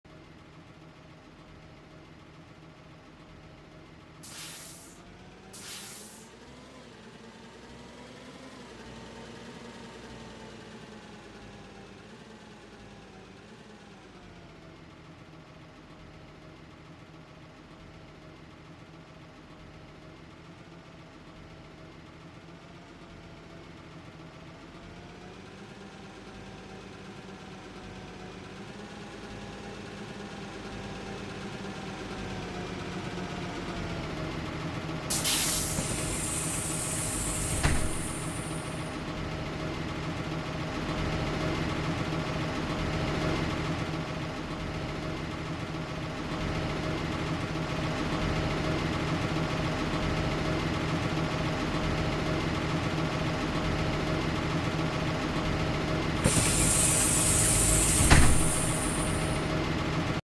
Bus 1811 Pulling into Brookhaven high school [ON 1210'S ROUTE]
2019 Thomas HDX w/ B6.7 Cummins

Fun fact: this is @Bus714YT old bus as a C2.